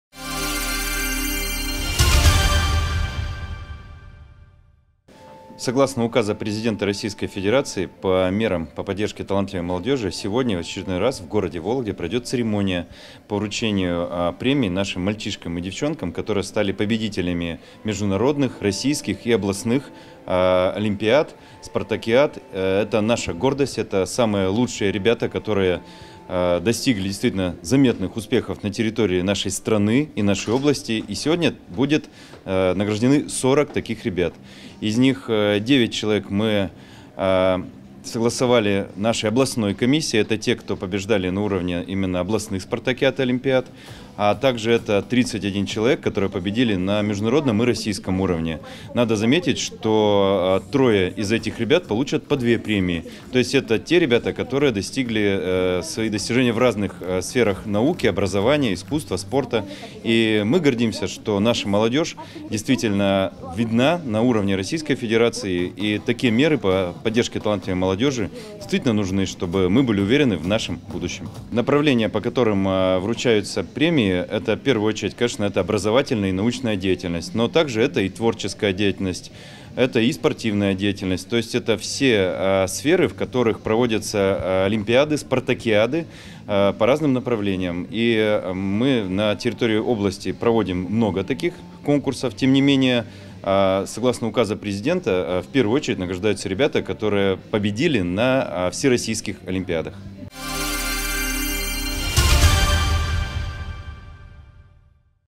Награждение лауреатов премии по господдержке талантливой молодежи
Вологодских лауреатов премии по государственной поддержке талантливой молодежи наградили сегодня в Вологде. Среди них – победители региональных и всероссийских конкурсов из разных городов области (Вологды, Череповца, Тотьмы, Сокола и Устюжны). Они получили дипломы и денежные призы в размере 30 тысяч и 60 тысяч рублей. С заслуженной наградой ребят поздравил первый заместитель Губернатора Вологодской области Алексей Шерлыгин.